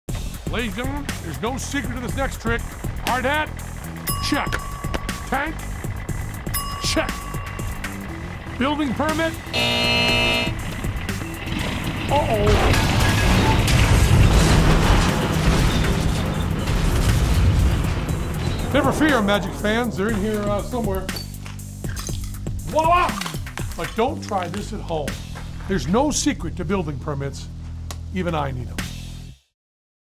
Building Permit Magic with Penn Jillette
There’s nothing magic about the need for building permits – even famed magician and Clark County resident Penn Jillette needs them.  The magician lent his talents to Clark County’s Building and Fire Prevention Department to help illustrate the need for building permits as the star of a new TV Public Service Announcement making its debut for Building Safety Month.  The PSA was filmed on location when Jillette was demolishing is former homed, called “The Slammer,” in the southwest valley as part of a movie project.